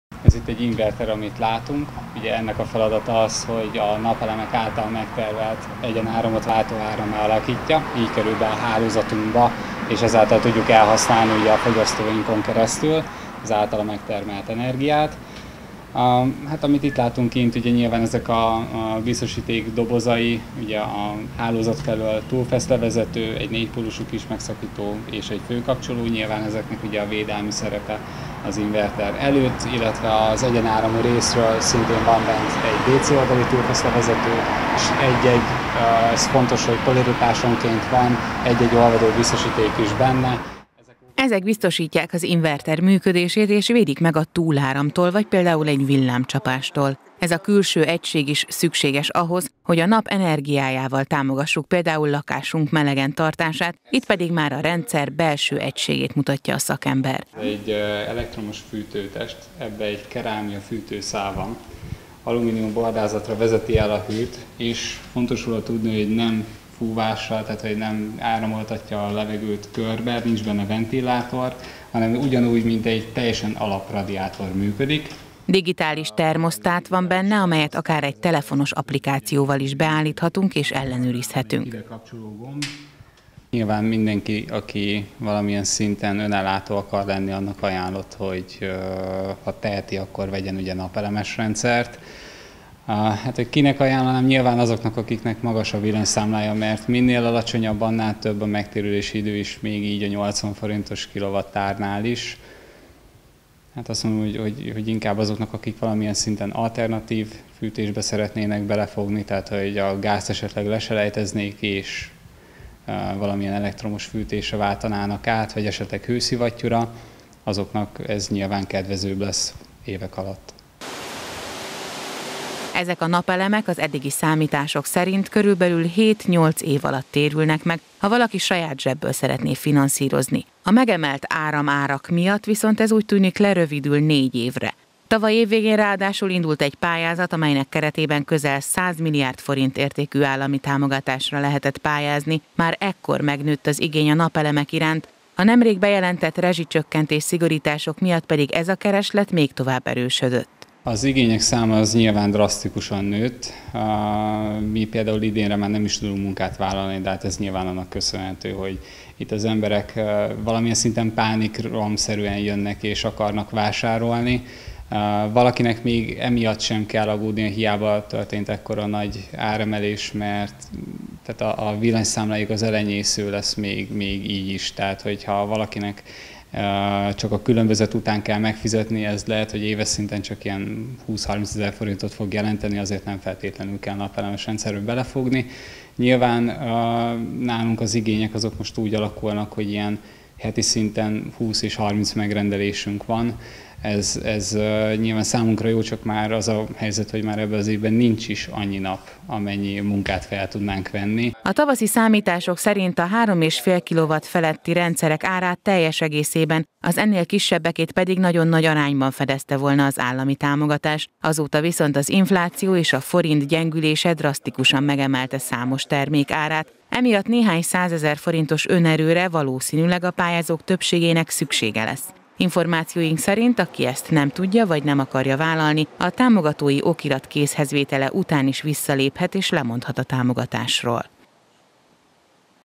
Óriási a kereslet
Óriási a kereslet a napelemrendszerek iránt Szabolcs-Szatmár-Bereg megyében. Ennek oka, hogy az új villamosenergia árszabályozás miatt sokan szeretnék csökkenteni a foigyasztásukat és ezt elsősorban a napelemek felszerelésével kívánják megoldani. Tavaly egy 100 milliárd forint keretösszegű pályázatot hirdetett a kormányzat, amelyre rengeten jelentkeztek.